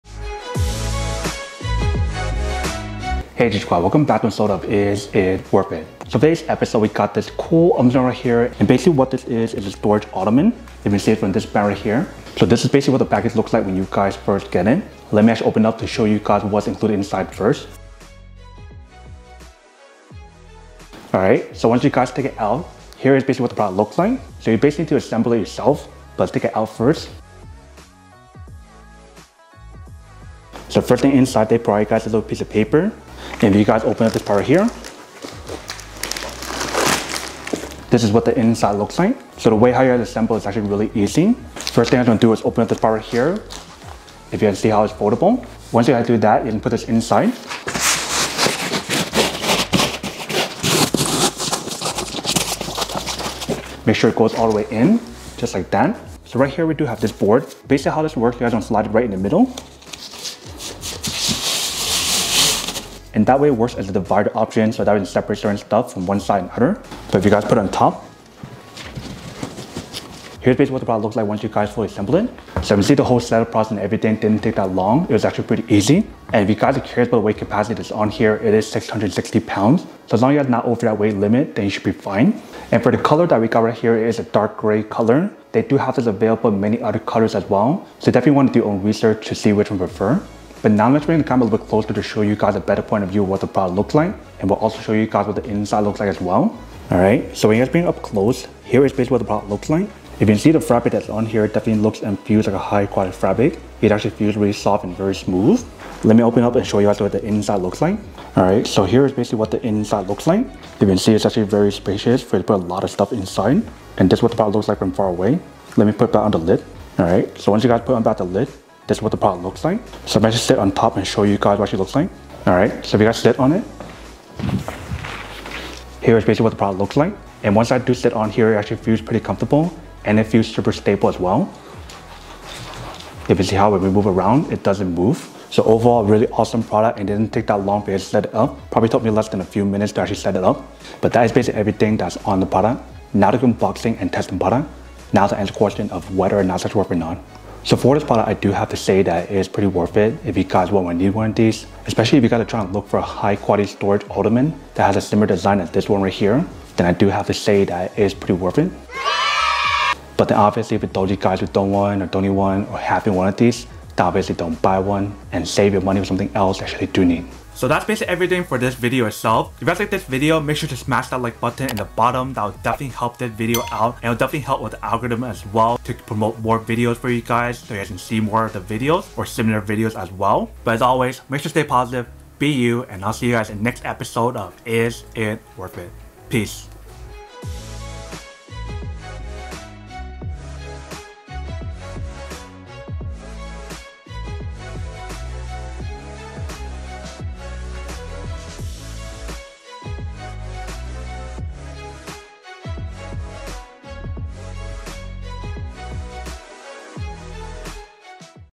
How to Use Songmics Folding Storage Ottoman Bench?
Are you in search for the best ottoman bench that you can use as storage? Check out the video to find out if this product is for you!

If you want:
Songmics Folding Storage Ottoman Bench